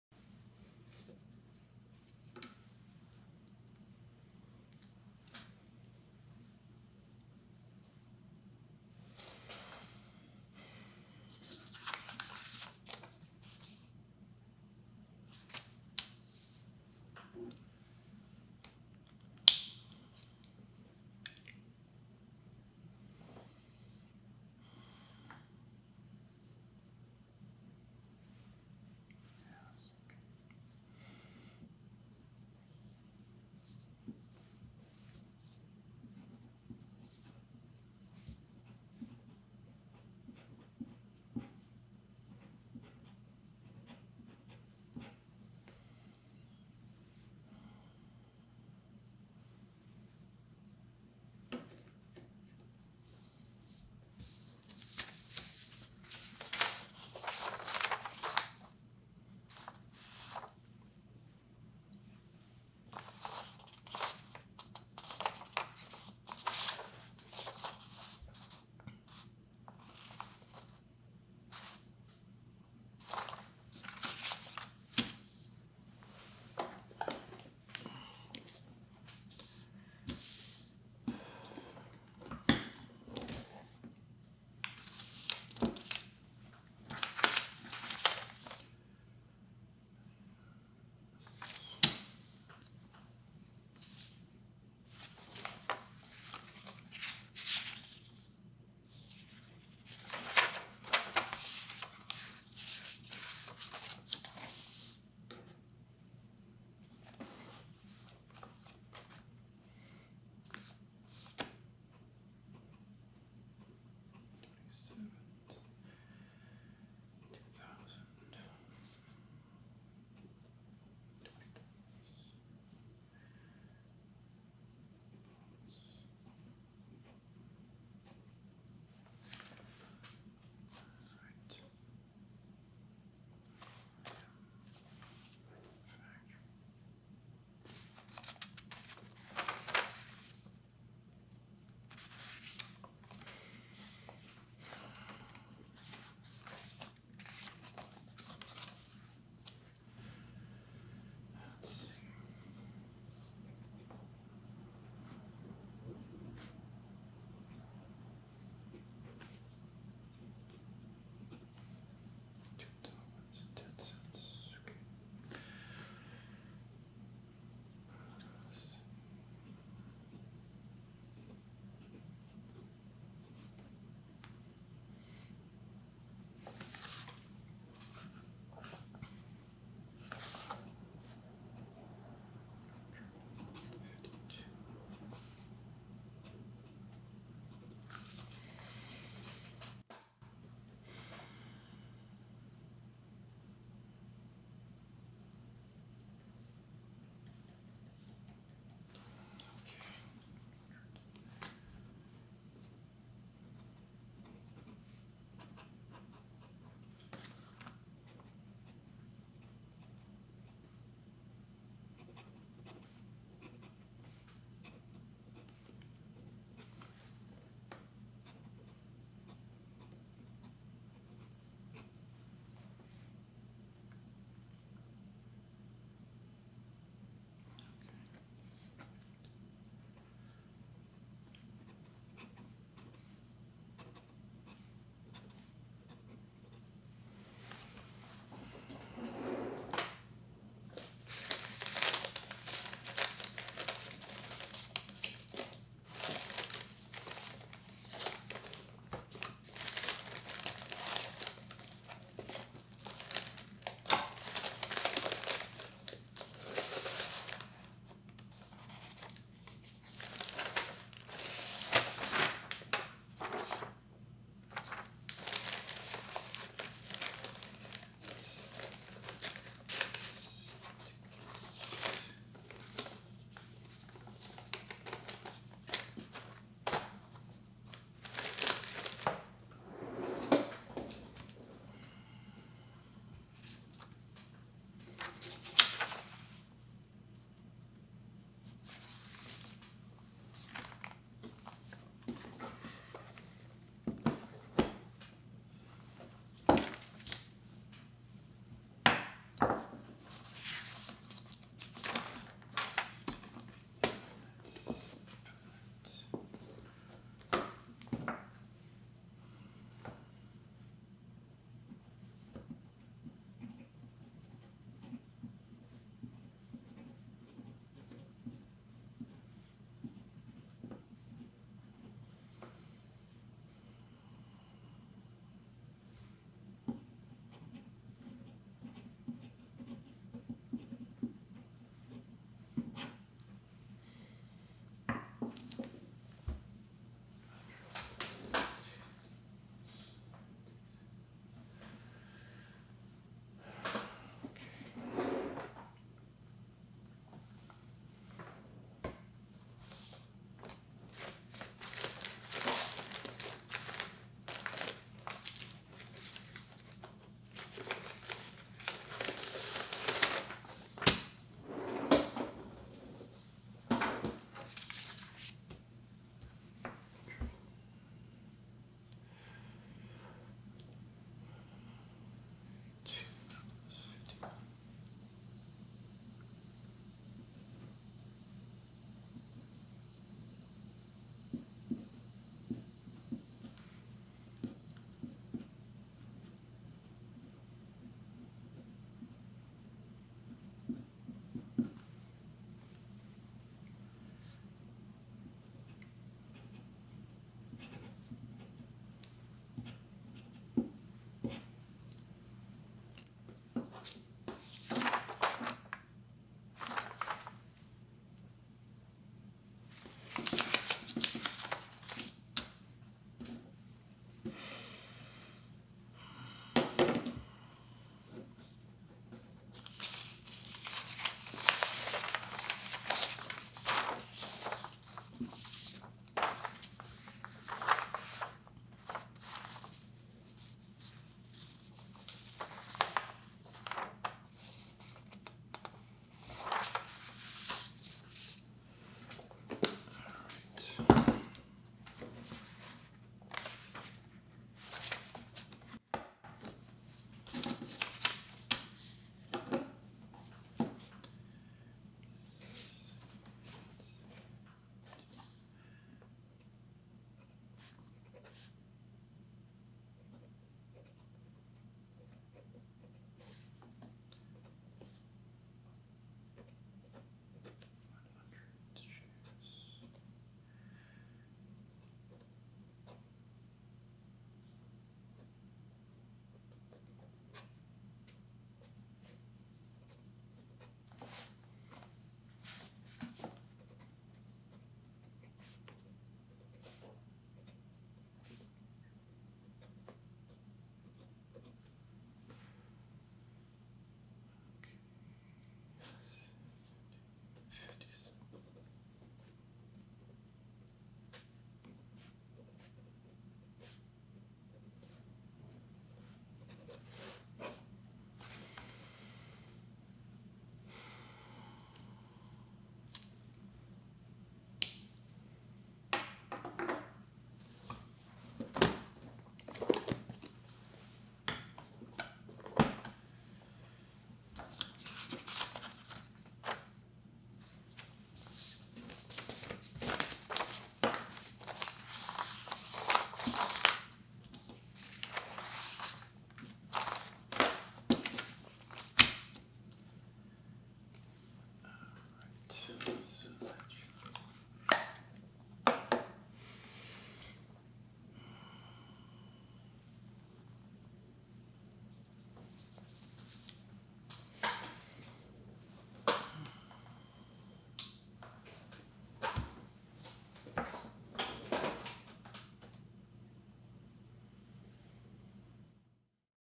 ASMR Fountain Pen writing by 1940s businessman working at his desk. (fountain pen, 1940s)
ASMR Fountain Pen writing by 1940s businessman working at his desk. (fountain pen writing sounds, 1940s style, no talking, 1940s set design)
3:10 close up of fountain pen
4:08 rummaging thru paperwork
5:00 flipping thru ledger
6:14 writing on certificate
8:29 putting top back on fountain pen
9:07 thinking and stroking beard